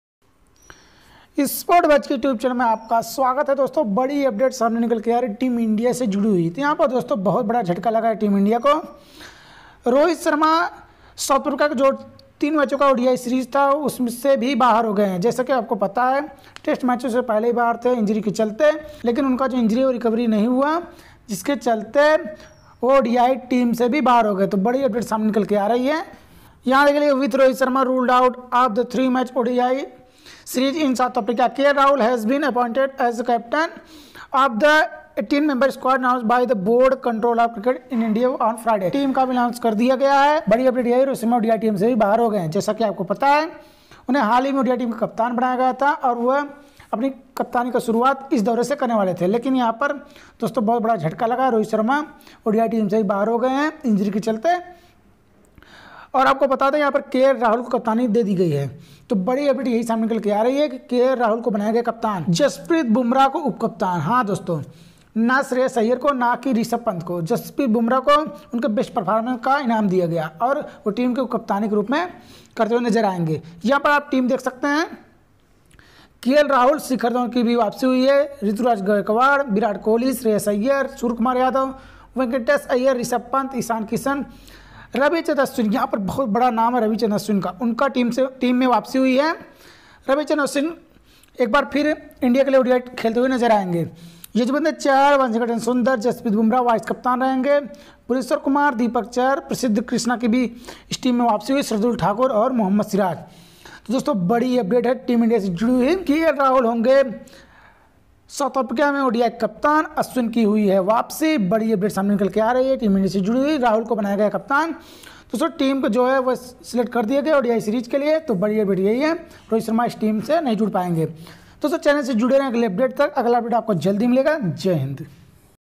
breaking news rohit sharma ruled out from South Africa odi series |||kl rahul odi captain for ind
india vs sa odi series